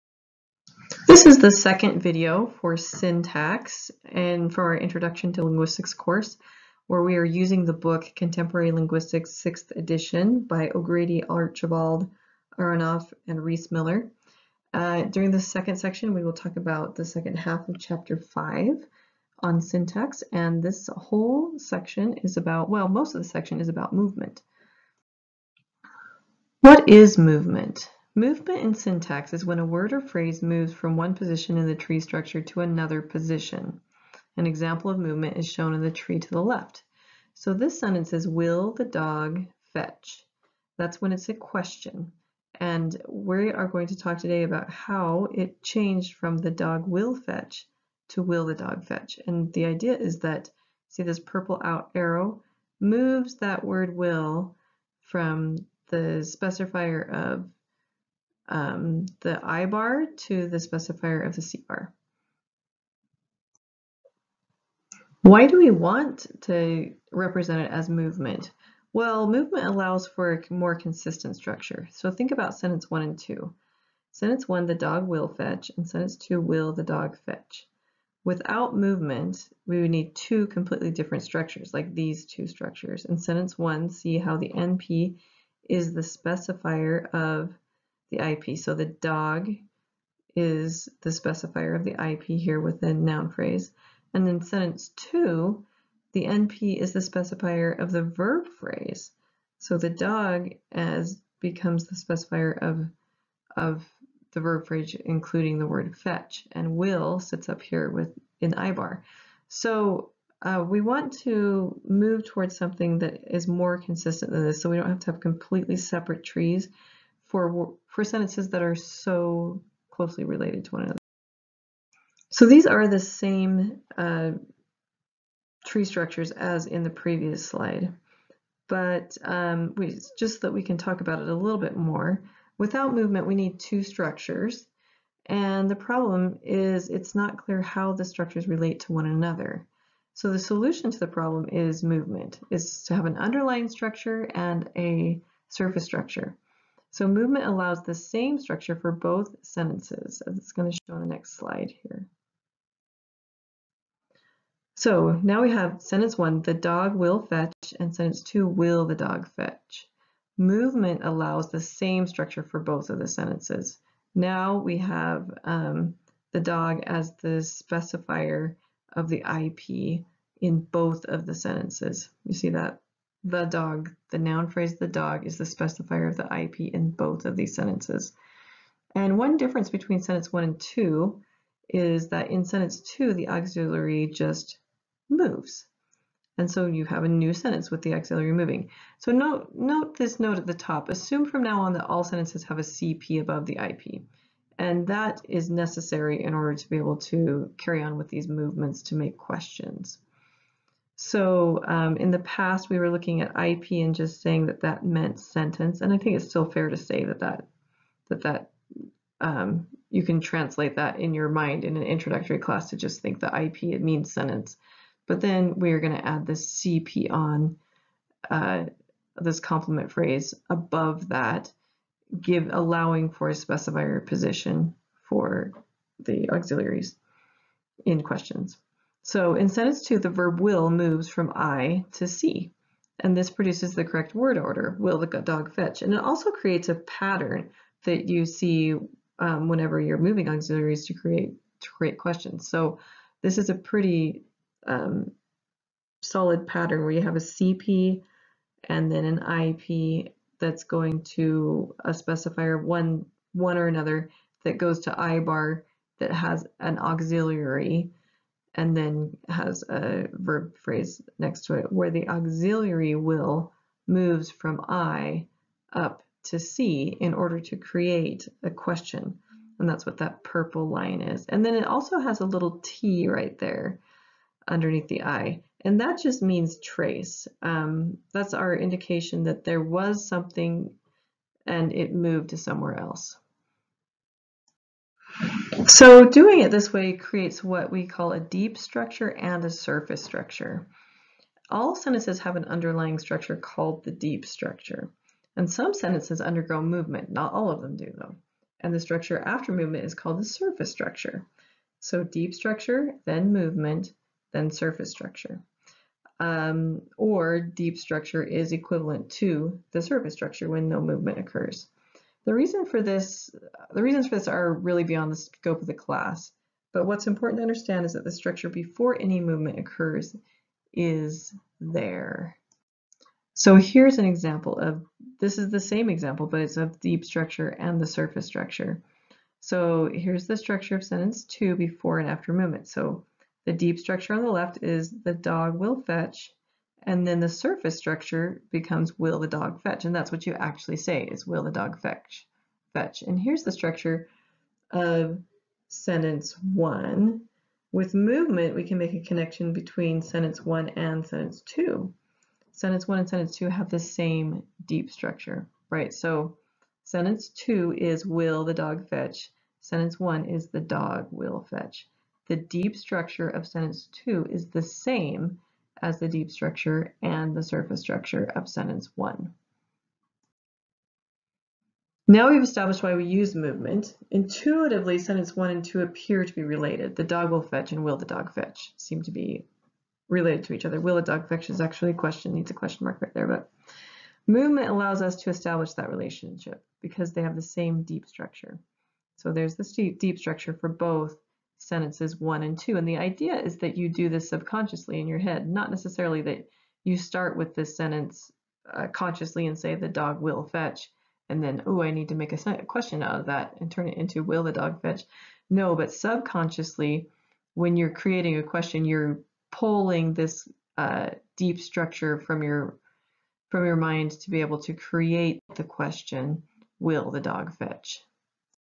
Syntax Two
The second of two videos to supplement chapter 5: Syntax from the textbook Contemporary Linguistics, 6th Edition by William O'Grady, John Archibald, Mark Aronoff, and Janie Rees-Miller. Power Point slides by Cami Lewandowski and Presentation by Tiffany Buckingham Barney